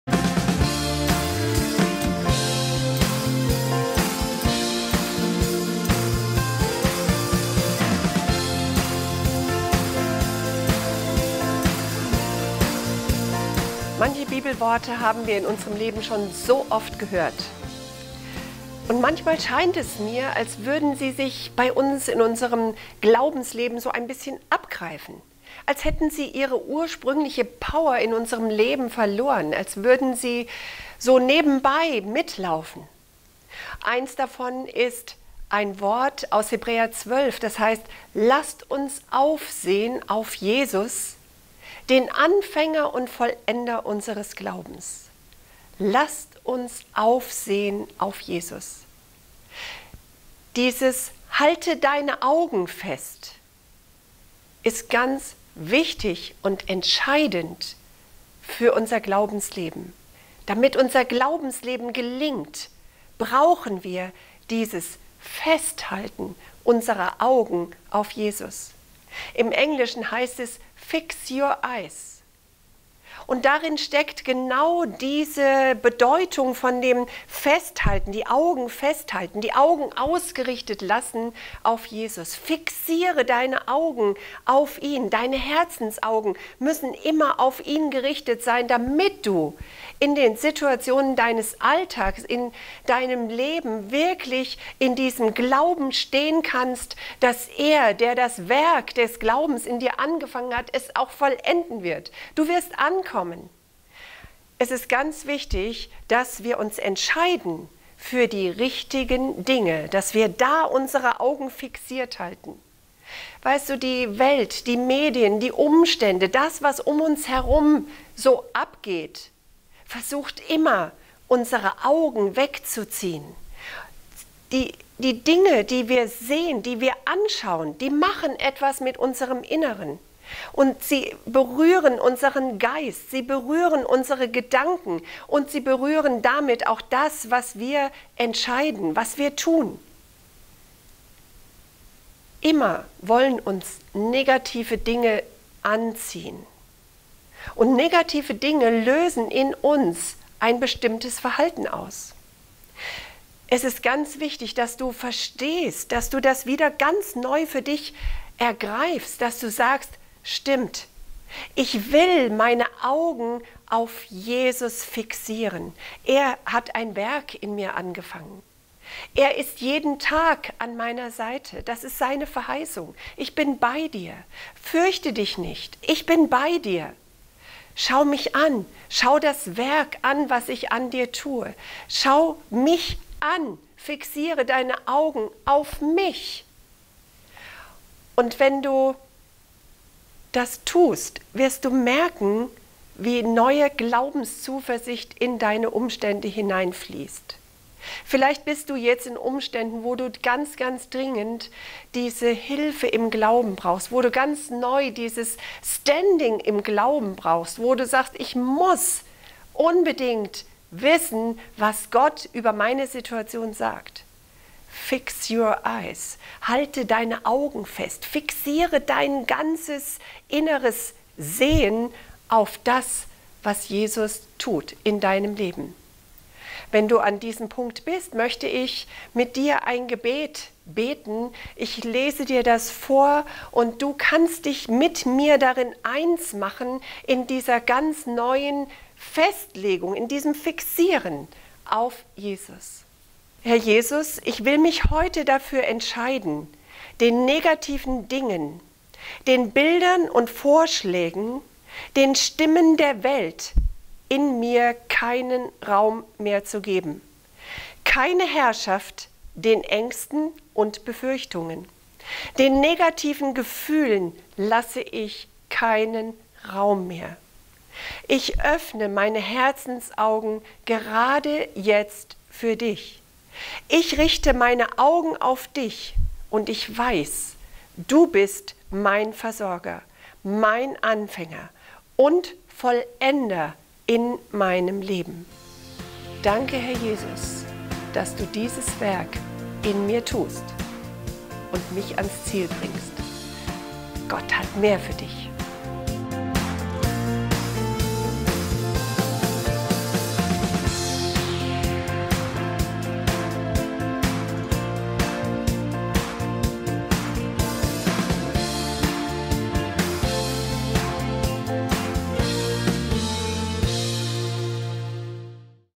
Den Blick fixieren
Sind deine Augen heute fest auf Jesus gerichtet? Es ist so leicht, in dem Destruktiven der Welt um uns herum und unter den Herausforderungen des Tages den Fokus zu verlieren. Was auch immer wir mit unseren Augen „fixieren“, wird unseren Geist für diese Sache öffnen. Was immer wir mit unseren Augen betrachten, wird eine Reaktion in uns hervorrufen.

Wir müssen uns bewusst dazu entscheiden, unsere Augen auf Jesus gerichtet zu halten, weg von den Umständen der Welt oder den Dingen, die uns bedrängen und überwältigen wollen.

Was brauchst du heute in deinem Leben?
Heilung? Richte deine Augen auf Jesus.
Befreiung, Zuversicht, Stärke? Richte deine Augen auf Jesus.

Du kannst dich von den anderen Dingen abwenden und deine Augen auf Jesus ausrichten. Erinnere dich: Er ist der Anfänger und Vollender deines Glaubens.

Ein Gebet für dich:
Herr Jesus,
ich will mich heute dafür entscheiden, den negativen Dingen, den Bildern und Vorschlägen, den Stimmen der Welt in mir keinen Raum mehr zu geben. Keine Herrschaft den Ängsten und Befürchtungen. Den negativen Gefühlen lasse ich keinen Raum mehr.
Ich öffne meine Herzensaugen gerade jetzt für Dich!
Ich richte meine Augen auf Dich und ich weiß, Du bist mein Versorger, mein Anfänger und Vollender in meinem Leben.
Danke, Herr Jesus, dass Du dieses Werk in mir tust und mich ans Ziel bringst.

Lasst uns auf Jesus sehen, den Anfänger und Vollender unseres Glaubens
Hebräer 12,2